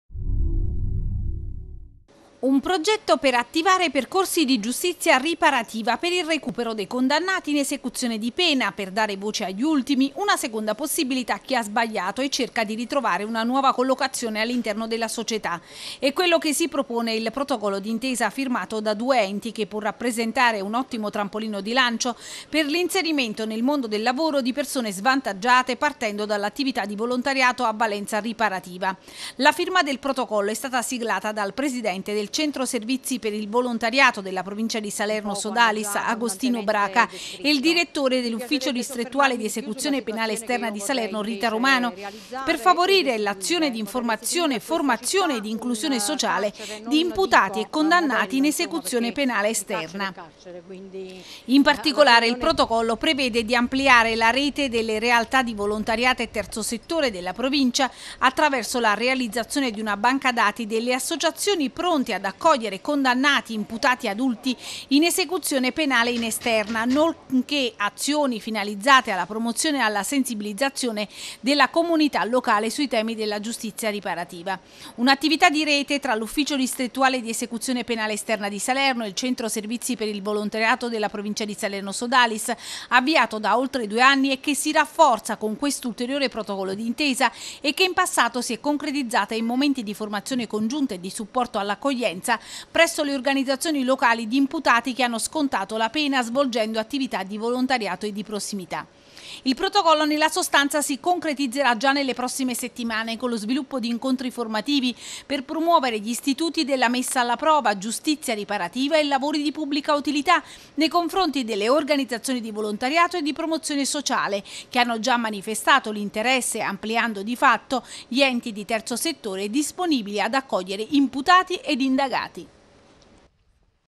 Salerno, Giustizia riparativa, firmato protocollo d’intesa
Firmato, dal presidente del Centro Servizi per il volontariato della provincia di Salerno Sodalis, Agostino Braca, e il Direttore dell’Ufficio Distrettuale di Esecuzione Penale Esterna di Salerno, Rita Romano, il protocollo di intesa   tra i due enti per favorire l’azione di informazione, formazione ed inclusione sociale di imputati e condannati in esecuzione penale esterna.